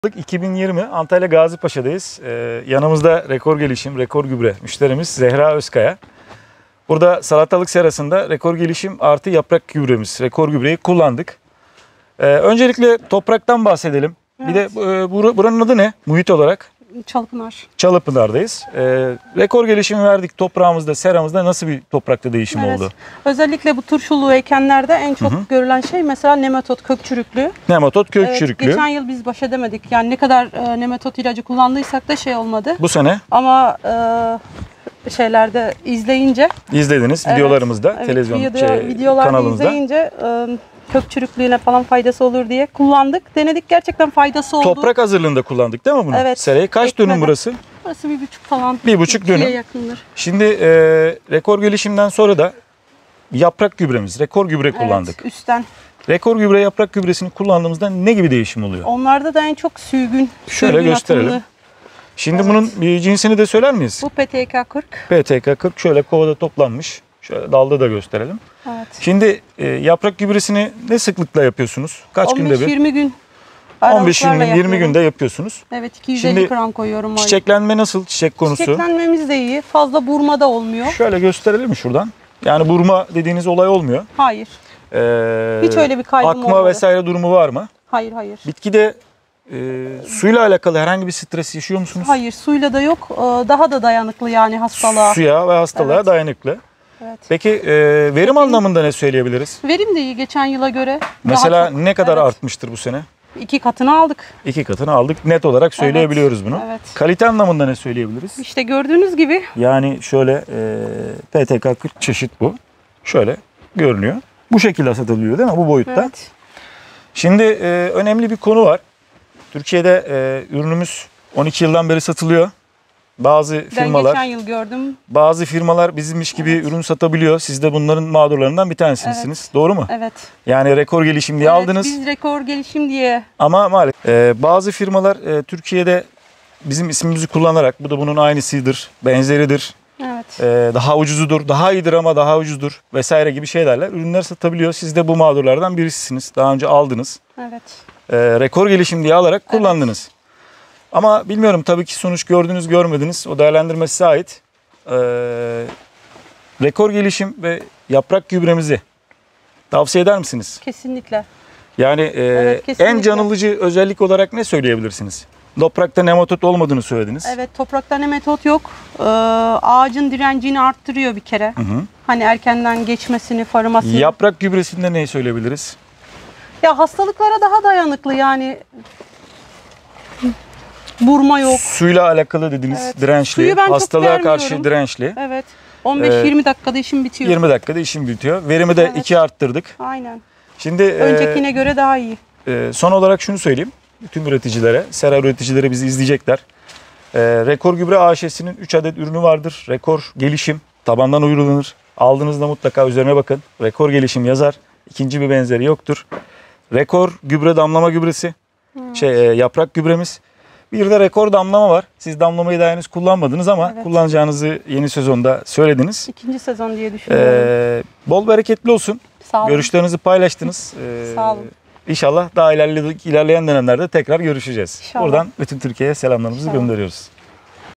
Antalya-Gazipaşa - Kesinlikle Tavsiye Ederim Salatalık Serasında Gübreleme -Benzeri Yok Bu Gübrenin
İzlemekte Olduğunuz Videoda Antalya Gazipaşa da Salatalık serasında REKOR GELİŞİM ve REKORGÜBRE YAPRAKGÜBRESİ uygulaması Yapılmıştır,  Müşterimiz piyasada Rekor gelişim sattığını iddia eden bir firmadan buda rekor gelişim aynısı diyerek ürün satın almış ve madur edilmiştir, sonrasında tekrar rekor gelişim alarak serasın da istenilen verim ve kaliteyi sağlamıştır,Ürün satın alırken mutlaka üzerinde yazan firma markası logosu ve tescillerine bakalım rekorgelişim sadece yetkili bayilerinde satılmaktadır ve bir yan firması benzeri asla yoktur,